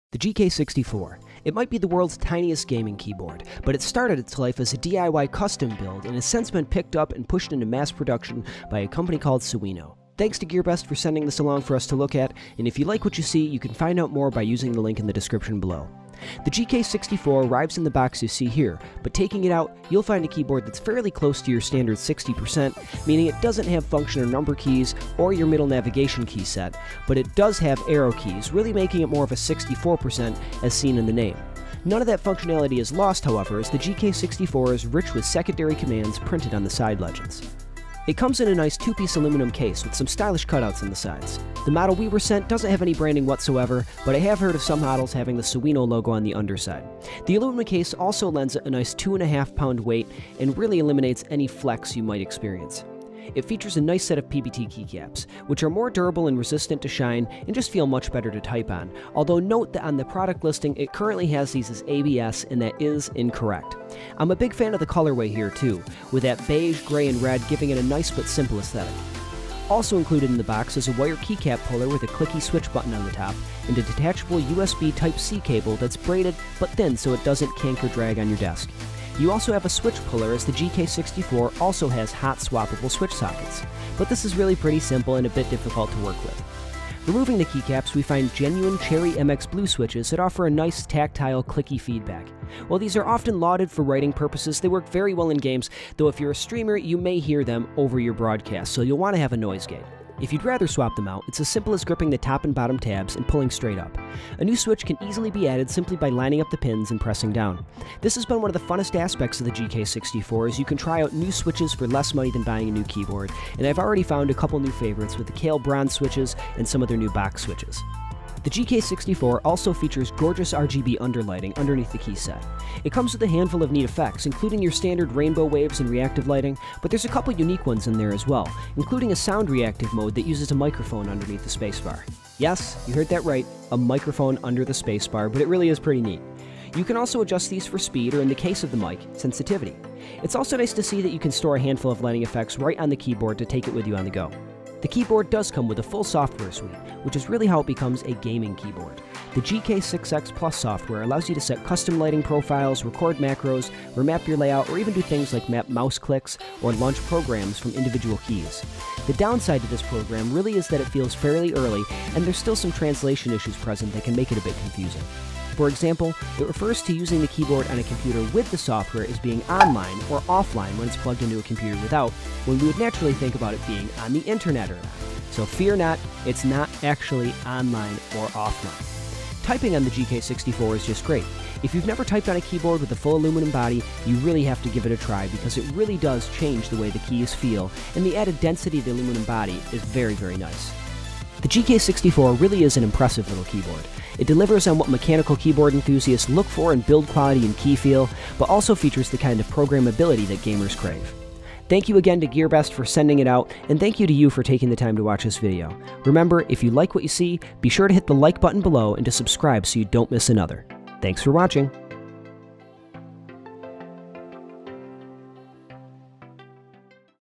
Sewino GK64 Review: The World's Smallest Gaming Keyboard?
Did we find the world's smallest mechanical gaming keyboard? Today, we're taking a look at the Sewino GK64 Mechanical Gaming Keyboard: a board that started its life as a DIY kit and was so beloved that Sewino picked it up for mass production. It's the best of both world between high-end enthusiast mechanical keyboards and gaming keyboards.

This keyboard is disappearing soon. Thankfully, there's one very close to it! Check that one out here:

See other mechanical keyboards at GearBest!